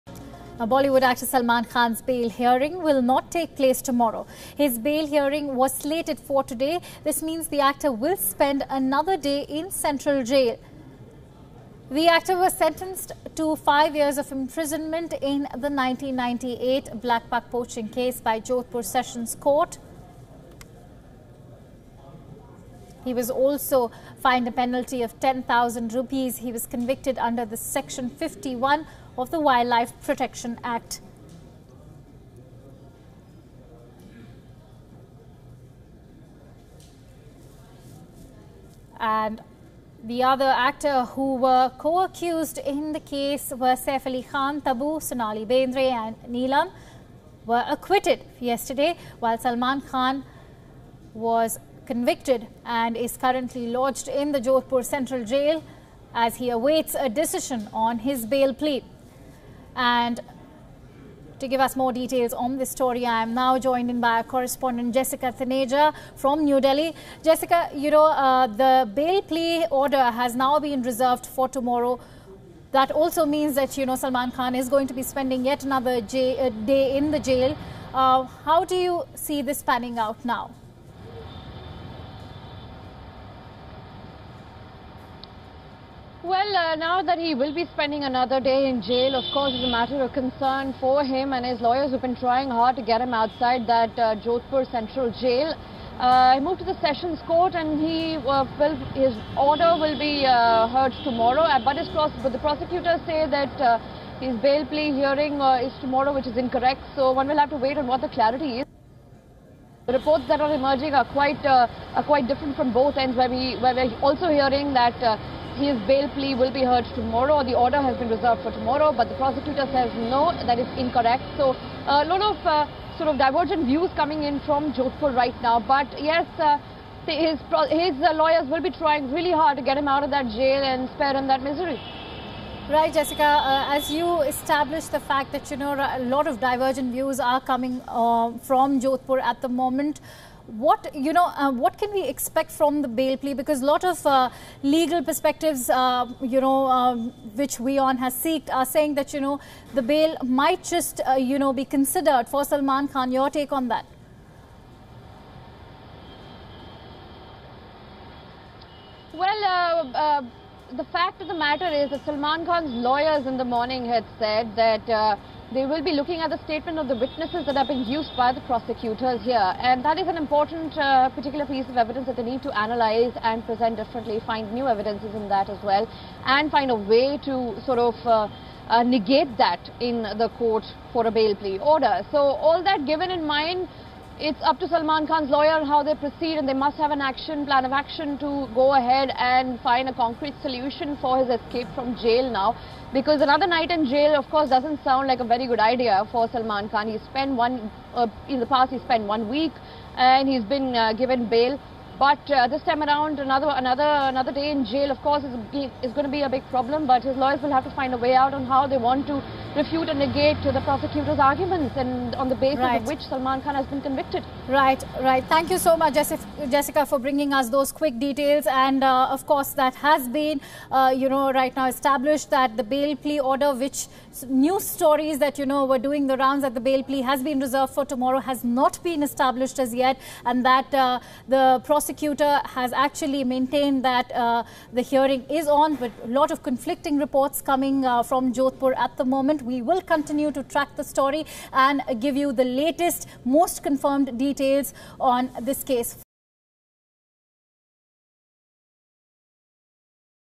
Jail, no bail: Salman Khan's trouble mounts as court reserves order till Saturday
Salman Khan will have to spend all of Friday in jail as well after Jodhpur Session's Court reserved bail order till Saturday. The actor was pronounced guilty of shooting two blackbucks in a case dating back to 1998, and was sentenced to five years in prison. Watch this videos to know.